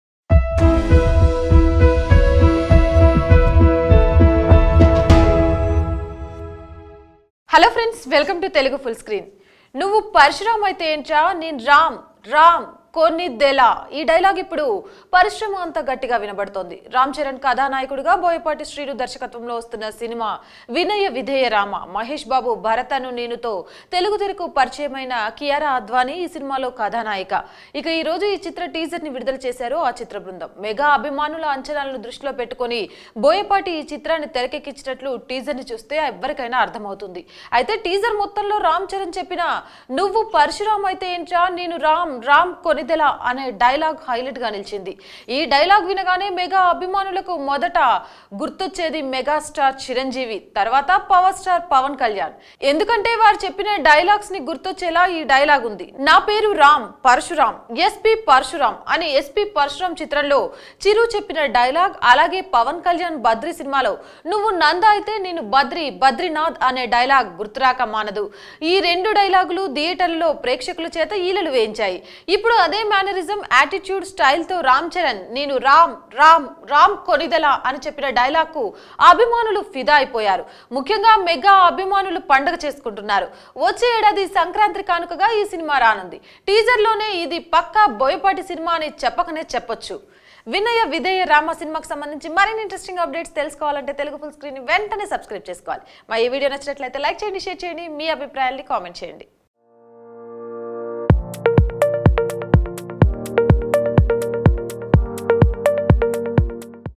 Ram Charan Once Again Remembers To Mega Star Chiranjeevi And Power Star Pawan Kalyan | TFS
Ram Charan Once Again Remembers To Mega Star Chiranjeevi And Power Star Pawan Kalyan. Vinaya Vidheya Rama Movie Is Directed By Boyapati Srinu And Produced By D.V.V.Danaiah. Music Is Composed By DSP. Ram Charan, Kiara Advani, Vivek Oberoi, Prasanna, Sneha Prasanna, Naveen Chandra, Ananya, Prashanth Thyagarajan, Aryan Rajesh, Madhu Nandan Are Playing Main Lead Roles In This Movie. Watch Latest Videos And For More Updates.

ENTERTAINMENT

MOVIES

NEWS

WEBSITES